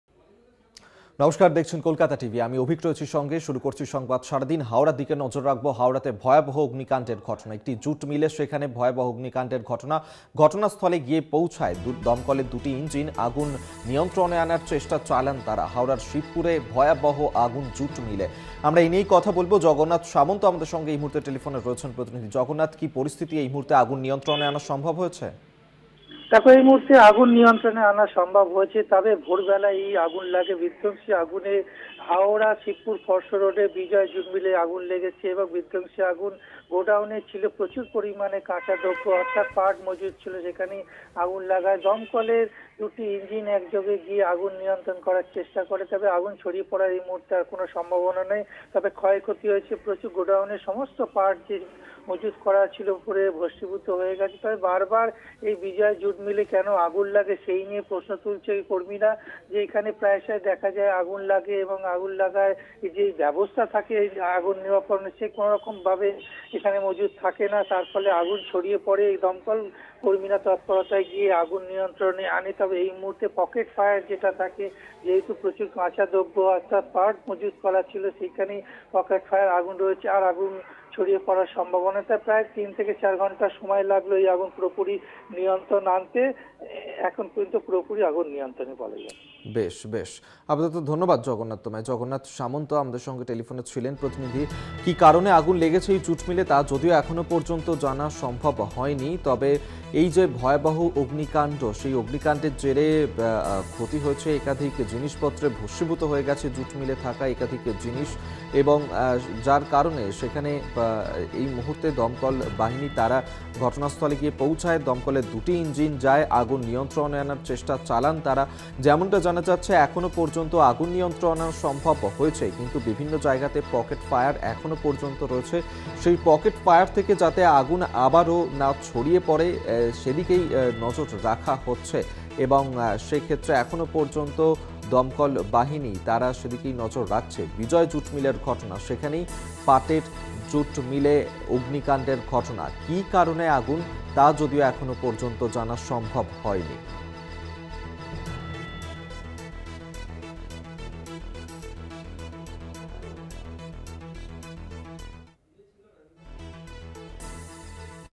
Howrah Fire | হাওড়ার শিবপুরে বিজয় জুট মিলের পাটের গোডাউনে ভয়াবহ আগুন
website link for this copy :
@Kolkata Tv